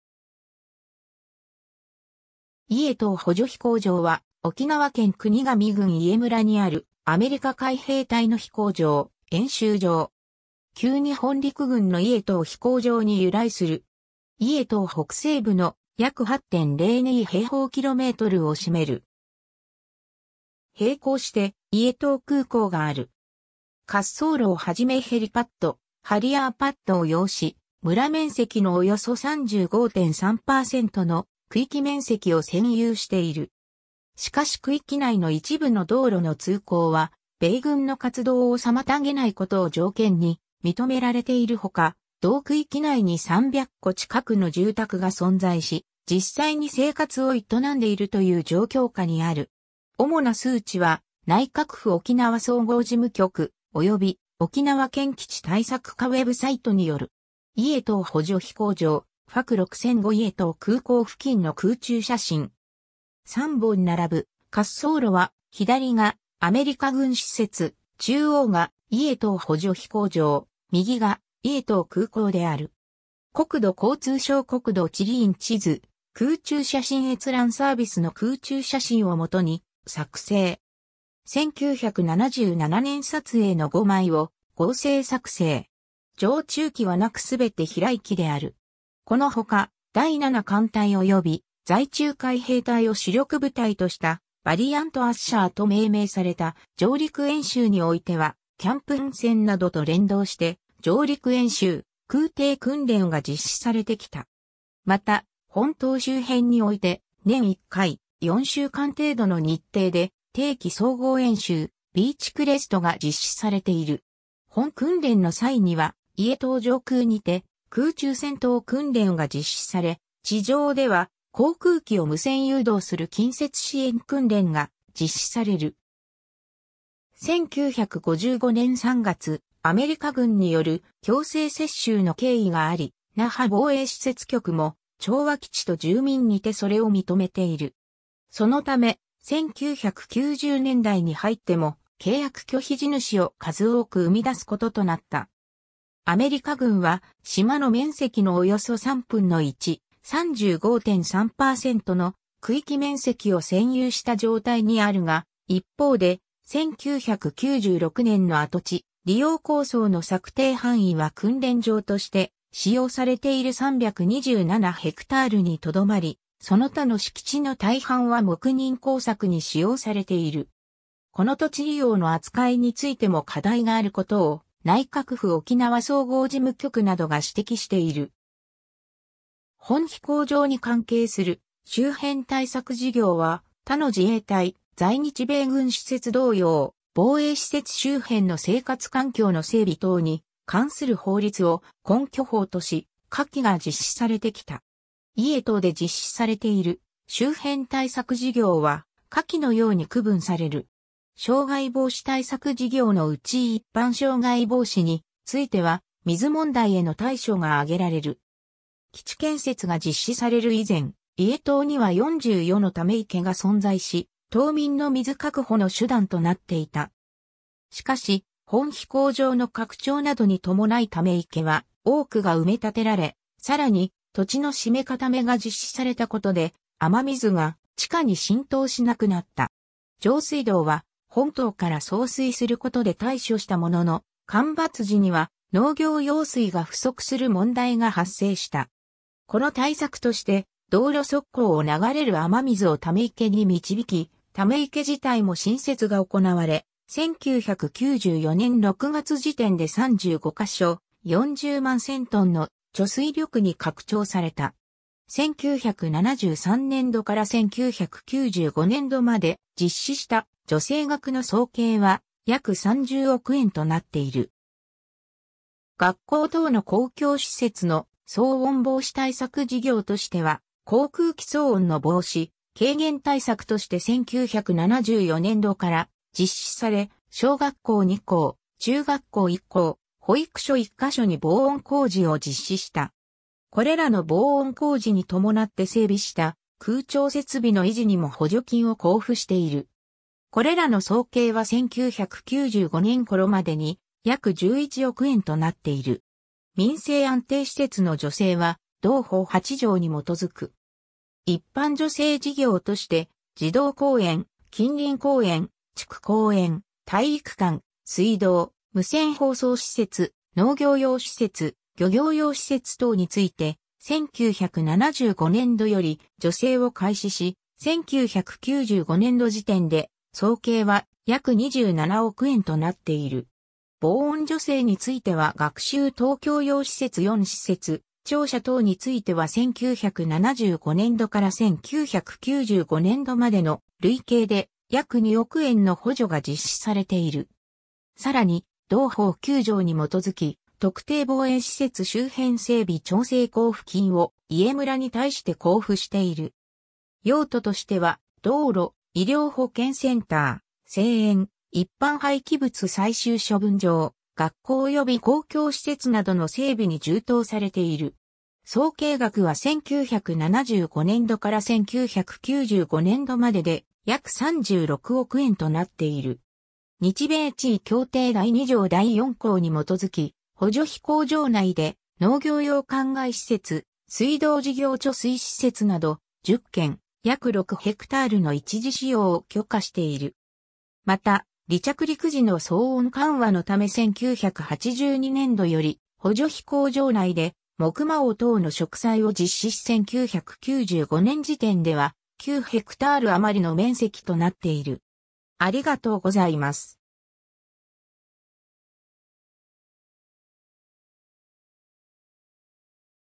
伊江島補助飛行場
伊江島補助飛行場（いえじまほじょひこうじょう Ie Jima Auxiliary Airfield）は、沖縄県国頭郡伊江村にあるアメリカ海兵隊の飛行場・演習場。
旧日本陸軍の伊江島飛行場に由来する。
 伊江島北西部の約8.02km2を占める。
平行して伊江島空港がある。
滑走路をはじめヘリパッド、ハリアーパッドを擁し、村面積のおよそ35.3%の区域面積を占有している。
しかし区域内の一部の道路の通行は米軍の活動を妨げないことを条件に認められているほか、同区域内に300戸近くの住宅が存在し、実際に生活を営んでいるという状況下にある。
 主な数値は内閣府沖縄総合事務局、及び沖縄県基地対策課ウェブサイトによる。
 伊江島補助飛行場 (Ie Jima Auxiliary Airfield) FAC6005 伊江島空港付近の空中写真。
3本並ぶ滑走路は、左がアメリカ軍施設、中央が伊江島補助飛行場、右が伊江島空港である。
1977年撮影の5枚を合成作成。
 常駐機は無く全て飛来機である。
 この他、第7艦隊及び在沖海兵隊を主力部隊とした「バリアント・アッシャー」と命名された上陸演習においてはキャンプ・ハンセンなどと連動して上陸演習、空挺訓練が実施されてきた。
 また、本島周辺において年1回、4週間程度の日程で定期総合演習「ビーチ・クレスト」が実施されている。
本訓練の際には伊江島上空にて空中戦闘訓練が実施され、地上では航空機を無線誘導する近接支援訓練が実施される。
 1955年3月、アメリカ軍による強制接収の経緯があり、那覇防衛施設局も『調和 基地と住民』にてそれを認めている。
そのため、1990年代に入っても契約拒否地主を数多く生み出すこととなった。
アメリカ軍は島の面積のおよそ3分の1、35.3%の区域面積を占有した状態にあるが、一方で、1996年の跡地利用構想の策定範囲は訓練場として使用されている327haにとどまり、その他の敷地の大半は黙認耕作に使用されている。
この土地利用の扱いについても課題があることを内閣府沖縄総合事務局などが指摘している。
 本飛行場に関係する周辺対策事業は他の自衛隊・在日米軍施設同様「防衛施設周辺の生活環境の整備等に関する法律」を根拠法とし（以下本節で同法と呼ぶ）、下記が実施されてきた。
 伊江島で実施されている周辺対策事業は下記のように区分される。
 障害防止対策事業（同法3条に基づく）の内一般障害防止については、水問題への対処が挙げられる。
基地建設が実施される以前、伊江島には40余の溜池が存在し、島民の水確保の手段となっていた。
しかし、本飛行場の拡張などに伴い溜池は多くが埋め立てられ、更に土地の締め固めが実施されたことで雨水が地下に浸透しなくなった。
上水道は本島から送水することで対処したものの、旱魃時には農業用水が不足する問題が発生した。
この対策として、道路側溝を流れる雨水を溜池に導き、溜池自体も新設が行われ、1994年6月時点で35箇所、40万1000トンの貯水力に拡張された。
1973年度から1995年度まで実施した助成額の総計は約30億円となっている。
 学校等の公共施設の騒音防止対策事業としては、航空機騒音の防止・軽減対策として1974年度から実施され、小学校2校、中学校1校、保育所1箇所に防音工事を実施した。
これらの防音工事に伴って整備した空調設備の維持にも補助金を交付している。
これらの総計は1995年頃までに約11億円となっている。
 民生安定施設の助成は同法8条に基づく。
一般助成事業として、児童公園、近隣公園、地区公園、体育館、水道、無線放送施設、農業用施設（野菜類の出荷施設）、漁業用施設（漁港）等について、1975年度より助成を開始し、1995年度時点で総計は約27億円となっている。
 防音助成については学習等供用施設4施設、庁舎等については1975年度から1995年度までの累計で約2億円の補助が実施されている。
 更に、同法9条に基づき、特定防衛施設周辺整備調整交付金を伊江村に対して交付している。
用途としては道路、医療保健センター、聖苑（火葬場、斎場）、一般廃棄物最終処分場、学校及び公共施設などの整備に充当されている。
総計額は1975年度から1995年度までで約36億円となっている。
 日米地位協定第2条第4項(a)に基づき、補助飛行場内で農業用灌漑施設、水道事業貯水施設...